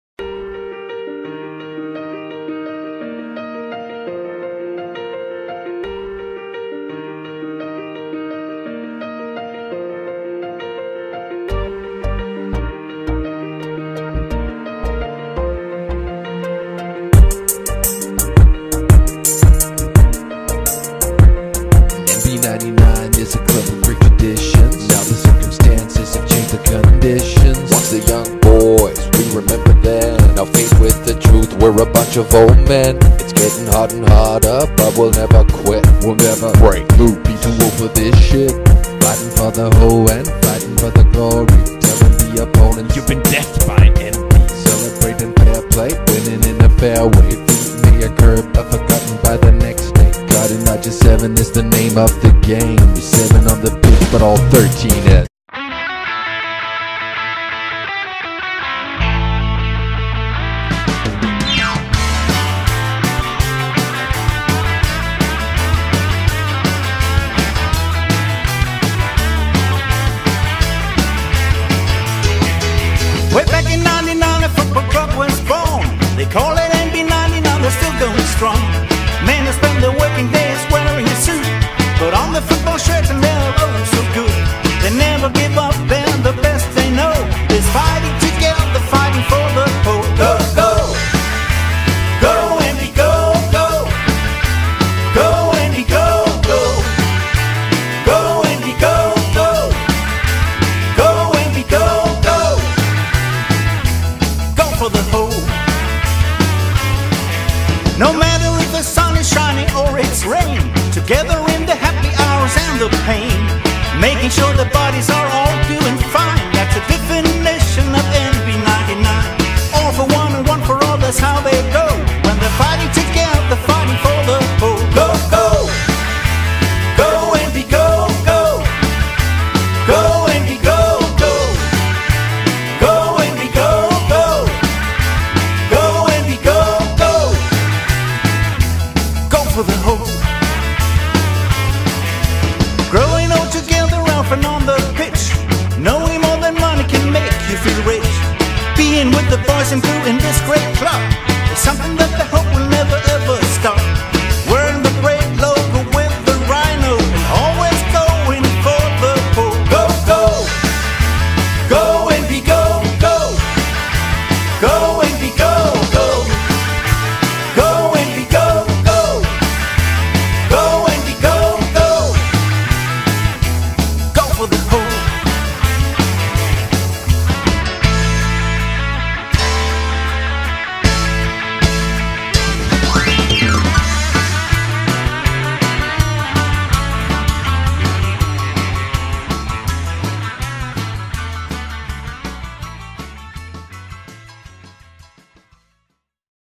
30. august 2014: NB 99 - USG
Præsentation af de 10 spillere, som NB 99 sender på banen på NB Arena.